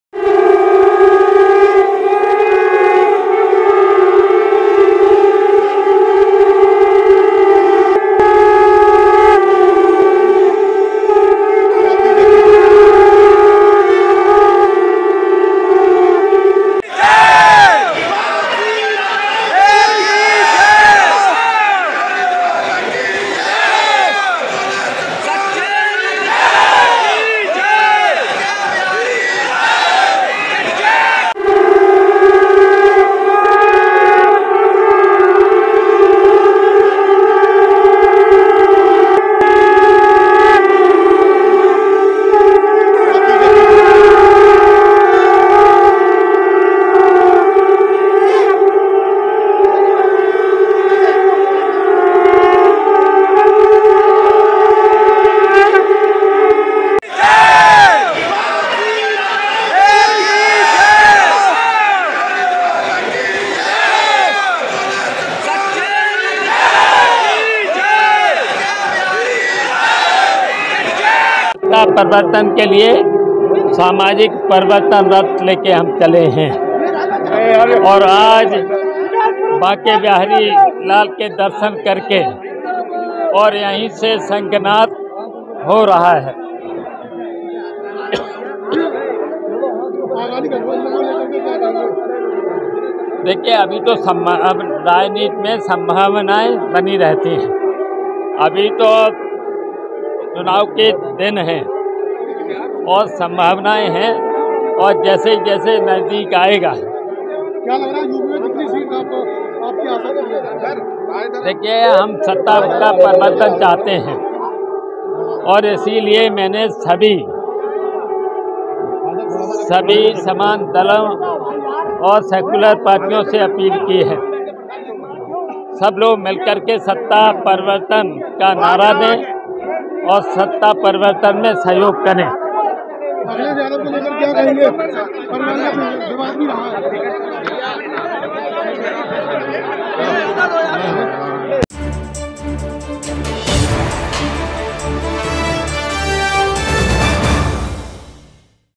भगवान बांके बिहारी के दर्शन कर शिवपाल ने किया 2022 के चुनाव का शंखनाद
उत्तर प्रदेश में मिशन 2022 को फतह करने के लिए आज बांके बिहारी के दर्शन को पहुंचे प्रगतिशील समाजवादी पार्टी के राष्ट्रीय अध्यक्ष शिवपाल यादव।  बांके बिहारी के चरणों में जाकर अपना चुवानी शंखनाद कर दिया है। जहां पर श्रद्धा और भक्ति के साथ बांके बिहारी की पूजा अर्चना की और जिसके बाद उन्होंने अपनी चुनावी रथ यात्रा की शुरुआत की। मंगलवार को प्रगतिशील समाजवादी पार्टी के राष्ट्रीय अध्यक्ष शिवपाल यादव बांके बिहारी के चरणों में अपनी जीत की कामना को लेकर पहुंचे। बांके बिहारी के सेवायत गोस्वामी द्वारा विधि विधान के साथ शिवपाल यादव को पूजा-अर्चना कराई। वहीं बांके बिहारी के प्रसाद बांके बिहारी के फूल माला और पटका पहनाकर उनका स्वागत किया।